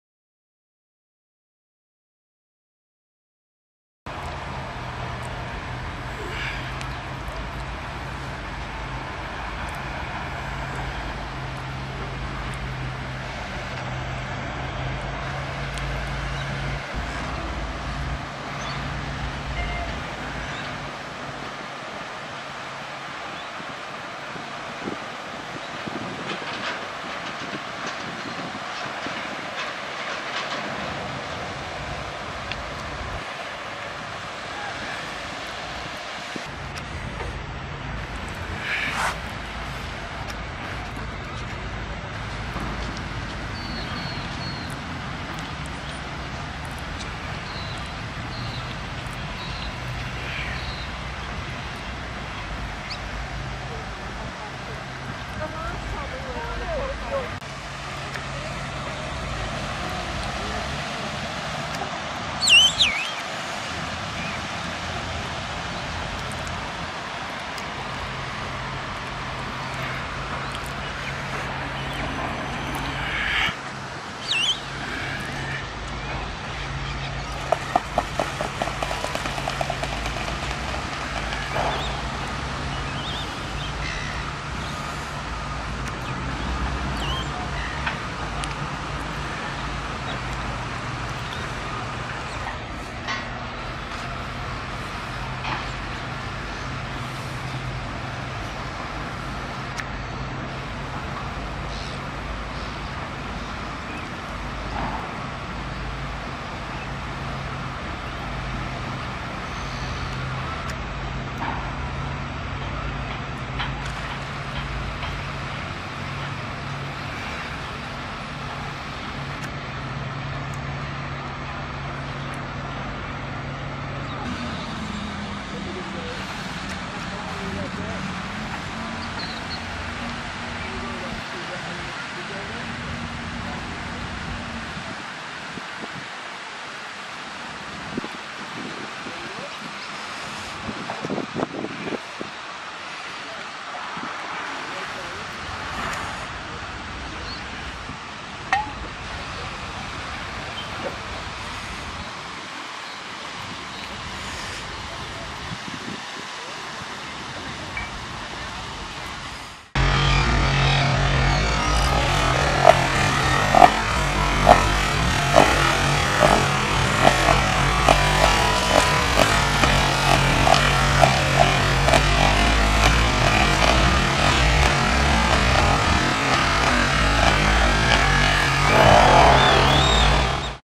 BLACK SWANS & 3 BABY
Black Swans and very protective mother at the river torrens in the Adelaide.

Film by Canon SX260 HS.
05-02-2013